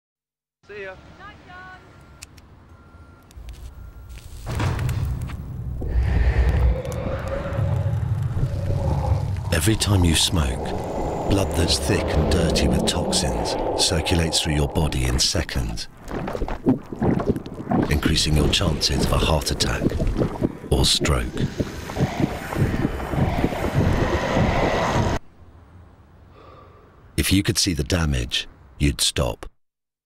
Eurgh...Ugh! Ew! Argh!
Brand spanking new and absolutely horrific PIF from Public Health England, under the NHS Smokefree campaign who seem to be running the show on smoking-related PIFs now. I don't know, whenever a new PIF gets released in Britain I just get so excited. I know that sounds incredibly sad, but there's been a sad lack of new material lately. We got a little bit of 'Moment of Doubt' over Christmas, no Fire Safety ones on telly (a couple of radio ones that I uploaded in the collection...) and in general, they just seem to be going into a lull of inactivity! It's such a shame for enthusiasts like myself, makes me wish I had my own little TARDIS so I could sit with my 9-year old self, eating jaffa cakes and watching Heartbeat on a Sunday night and go "Believe me, there will come a point where you'll actually enjoy watching these horrible little films."... but then again doesn't encountering your past self in time travel always result in some horrible paradox? ...Worth it.

Back on topic, blood-phobes and twitching-organ-phobes beware, you're in for a scare.

Scare Factor: 6/10
Absolutely Disgusting Factor: 9/10